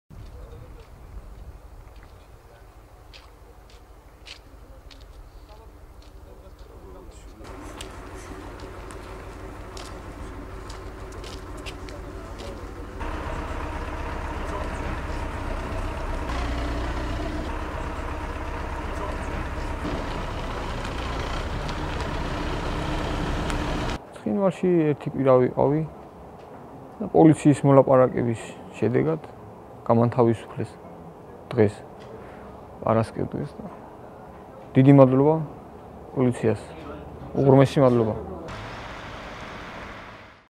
რუსი ოკუპანტების მიერ დაკავებული მოქალაქე გათავისუფლებულია
შსს-ს საინფორმაციო-ანალიტიკური დეპარტამენტის მიერ ცხელი ხაზის საშუალებით წარმოებული მოლაპარაკებების შედეგად გაათავისუფლეს რუსი ოკუპანტების მიერ, 20 დეკემბერს გორის რაიონის სოფელ კირბალთან, ე.წ. საზღვრის უკანონო კვეთის ბრალდებით დაკავებული ალექსანდრე შიუკაშვილი.

საინფორმაციო-ანალიტიკური დეპარტამენტის წარმომადგენლების აქტიური მოლაპარაკების შედეგად ასევე გათავისუფლდა 24 დეკემბერს სოფელ ახმაჯთან ე.წ. საზღვრის კვეთის ბრალდებით ასევე უკანონოდ დაკავებული, ახალგორის მაცხოვრებელი ანზორ ხუციშვილი.